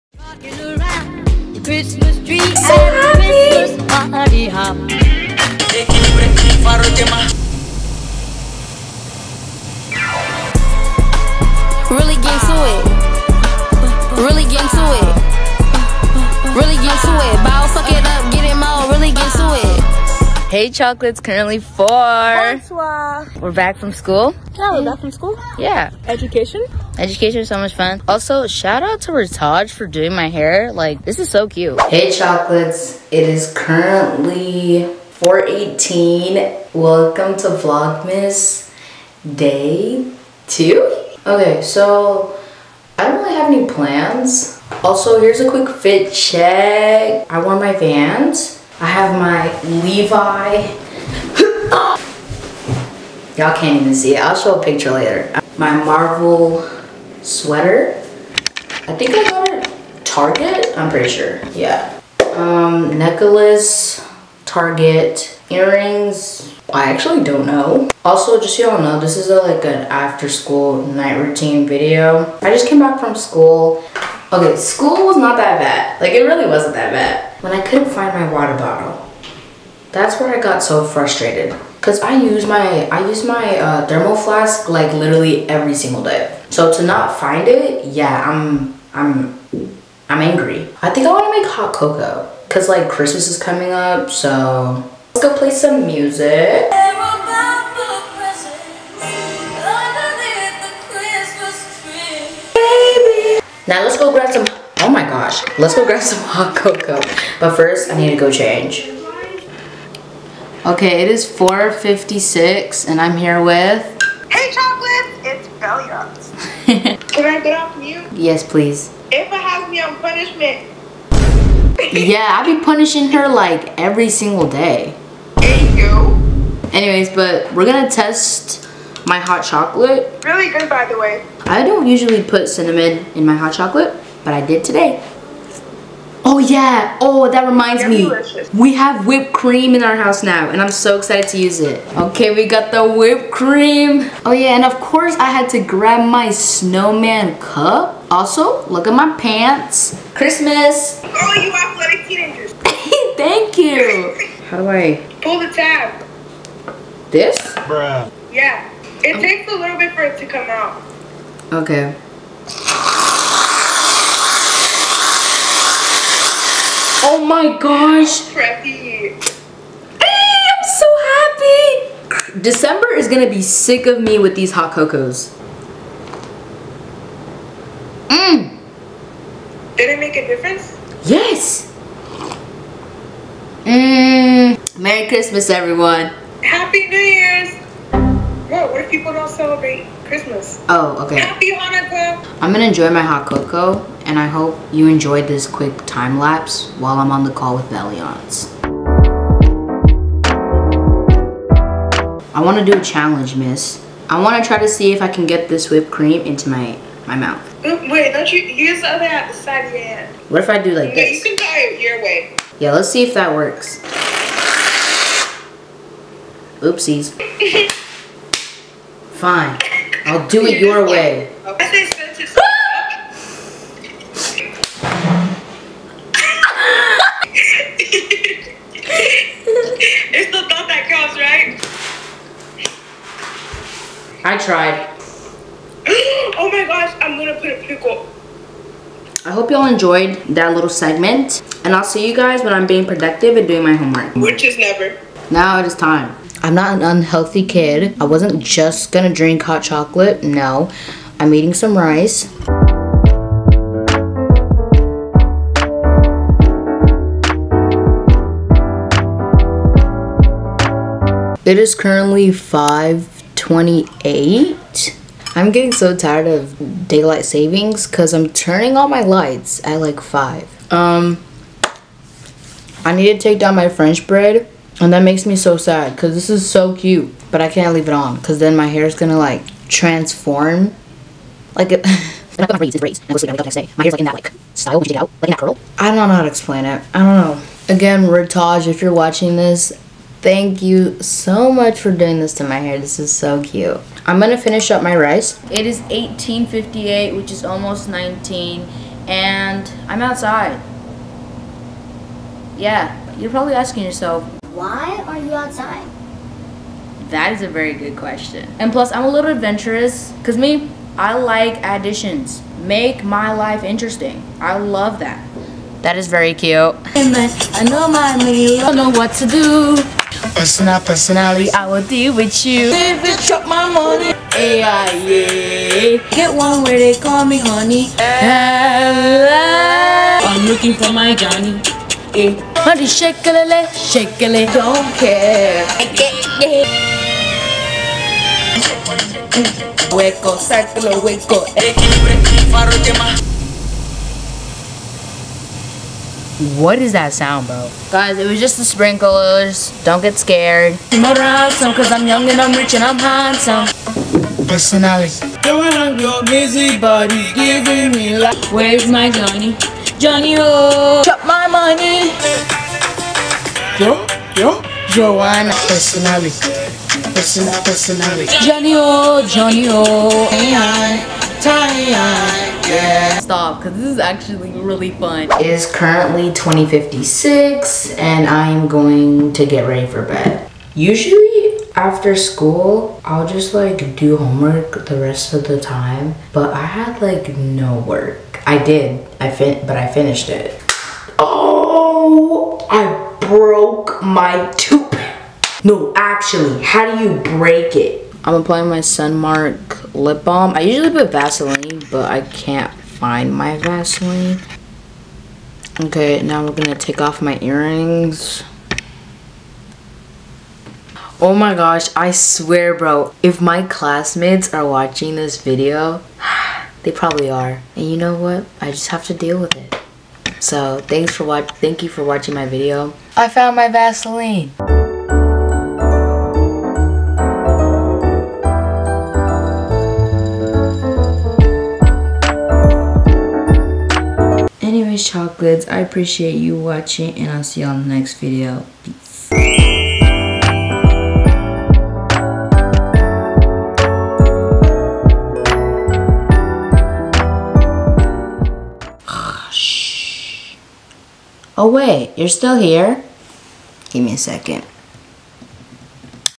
afterschool/night routine: Chocolate Vlogmas 02 - Eva Chocolate
Hey Chocolates, welcome to Chocolate Vlogmas! Where I record EVERYDAY until Christmas. This challenge wasn’t created by me; FACT: Vlogmas was created/started December 2, 2011. In this video, I’m doing my winter afterschool/night routine. I hope you enjoyed, happy winter!! ❄️